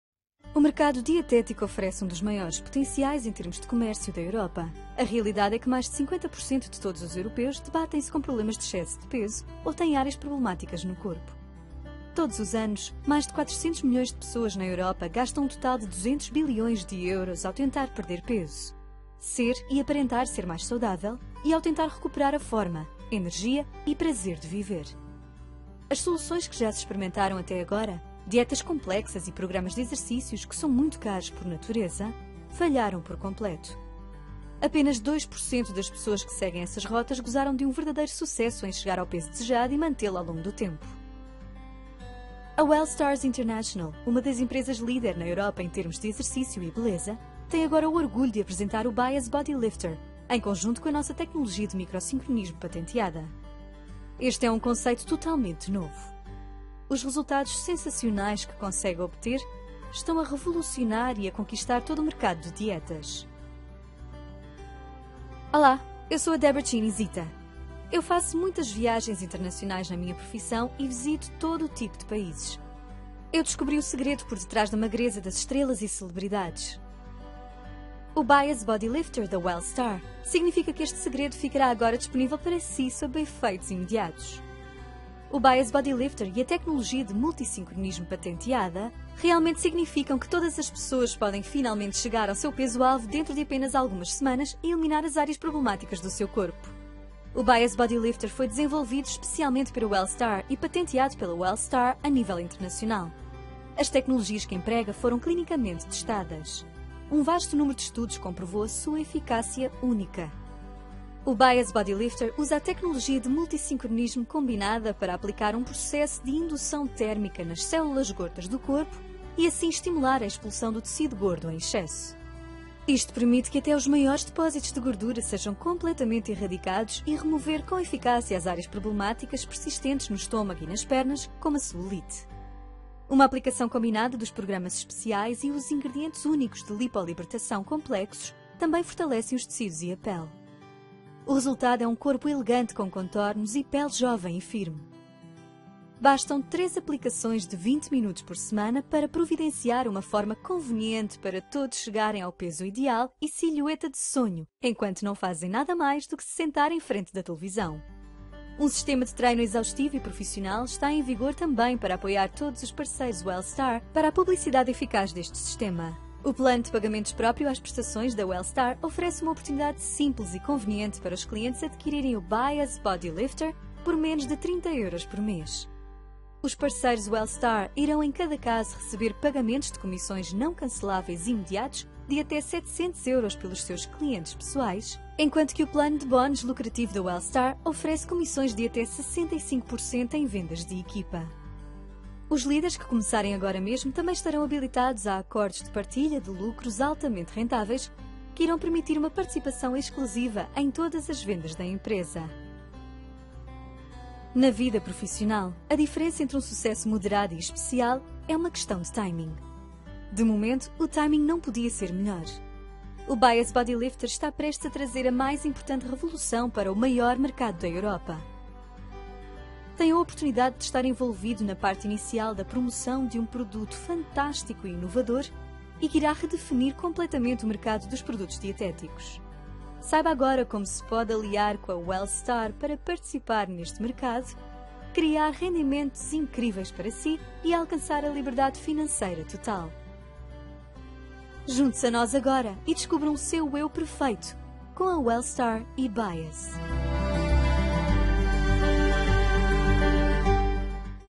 BYAS Body lifter da Wellstar by PPRO Team
O BYAS Body Lifter da Wellstar combina a tecnologia e a saúde e bem estar. Com a ajuda de rádio frequência permite-lhe obter a silhueta perfeita.

byas,
wellstar,
body lifter,